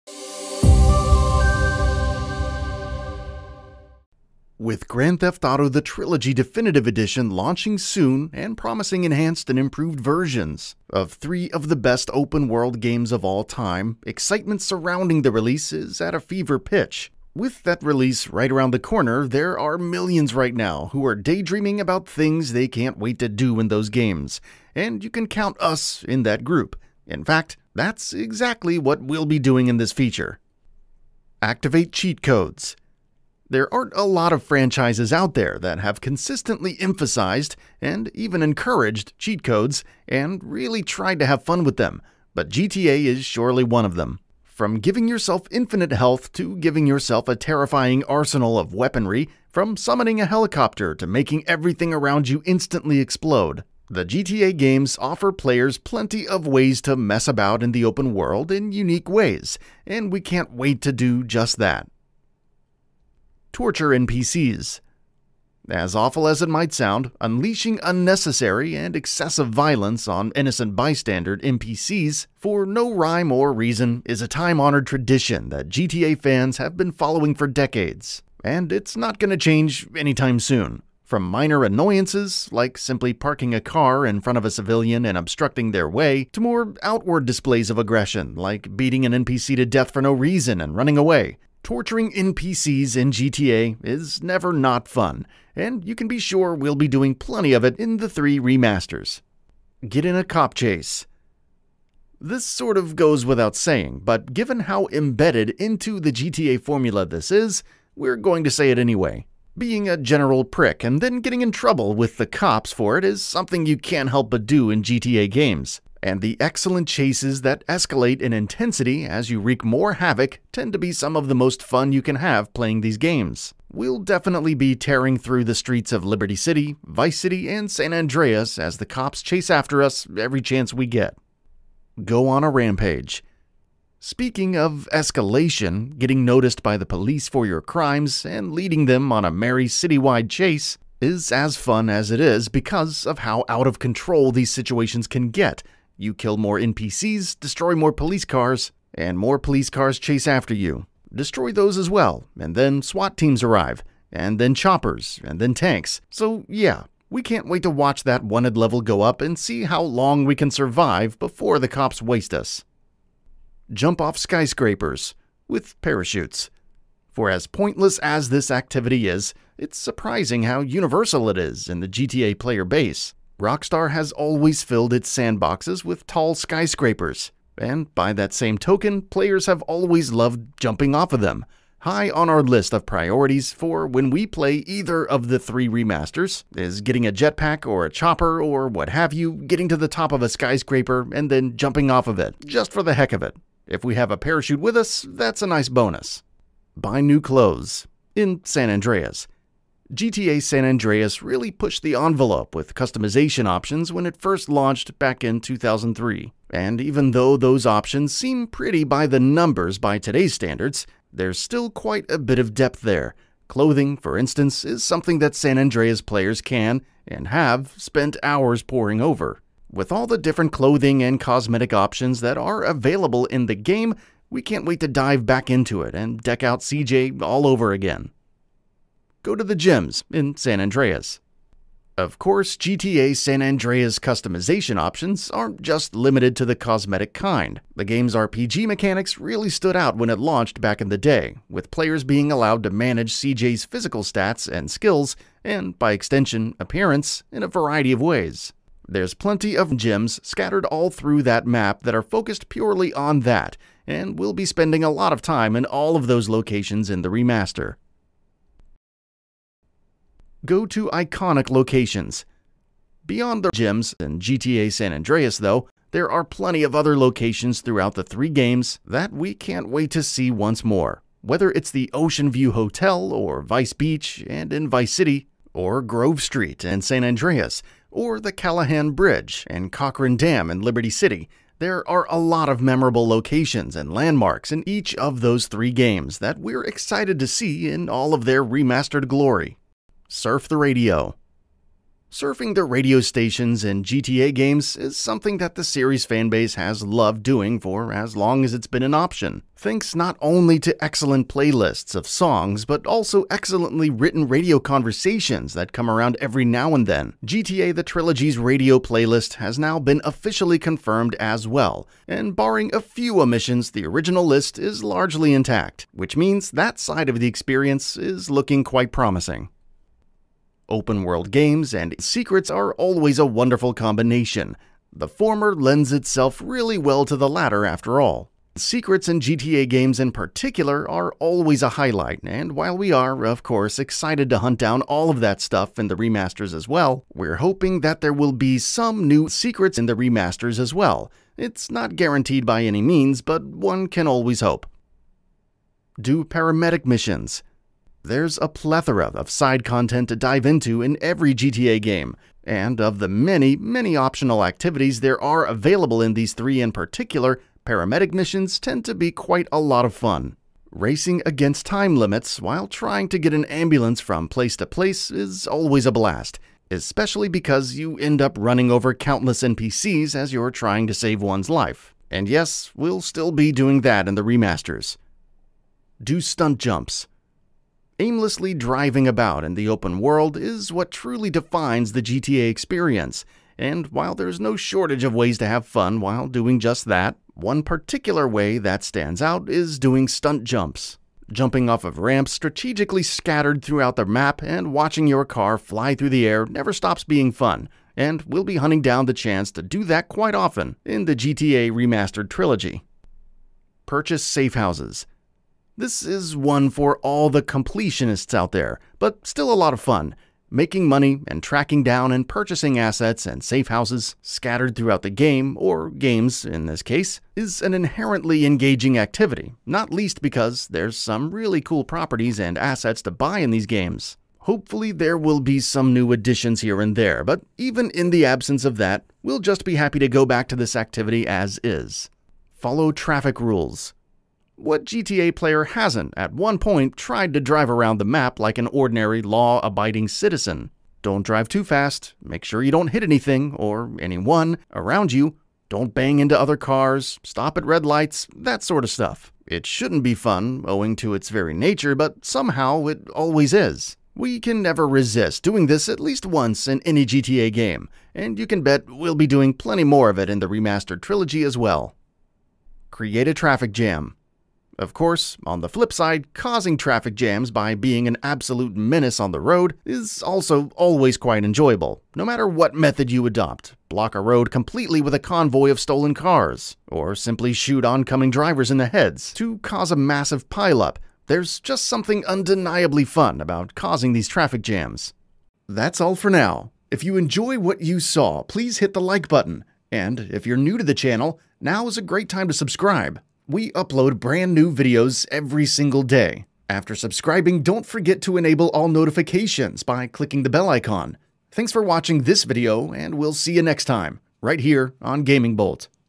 Grand Theft Auto Trilogy Remaster - 15 Things WE CAN'T WAIT TO DO
With Grand Theft Auto: The Trilogy - The Definitive Edition launching soon and promising enhanced and improved versions of three of the best open world games of all time, excitement surrounding the release is at a fever pitch.

With that release right around the corner, there are millions right now who're daydreaming about things they can't wait to do in those games- and you can count us in that group. In fact, that's exactly what we'll be doing in this feature.